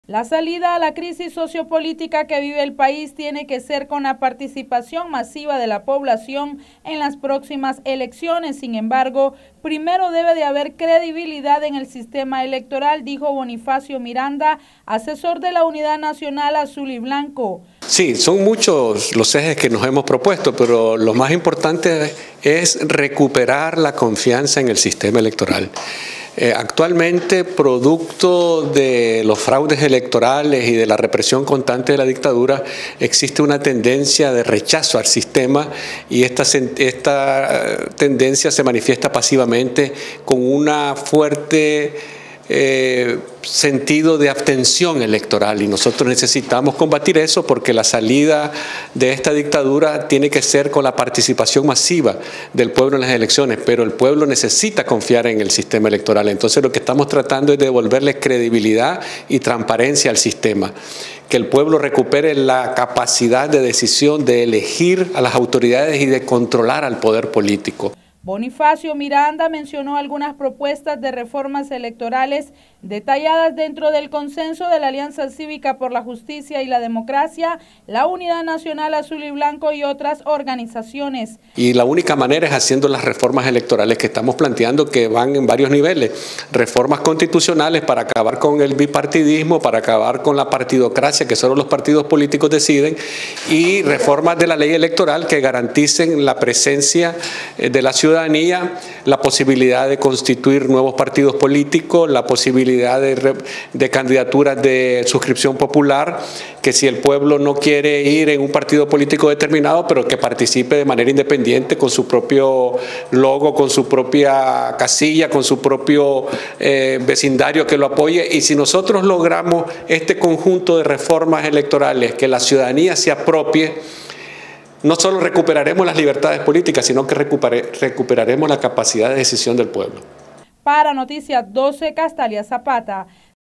“PRIMERO DE DEBE RESTAURAR LA CREDIBILIDAD EN EL SISTEMA ELECTORAL”
Primero se debe restaurar la credibilidad en el sistema electoral, dijo Bonifacio Miranda de la UNAB.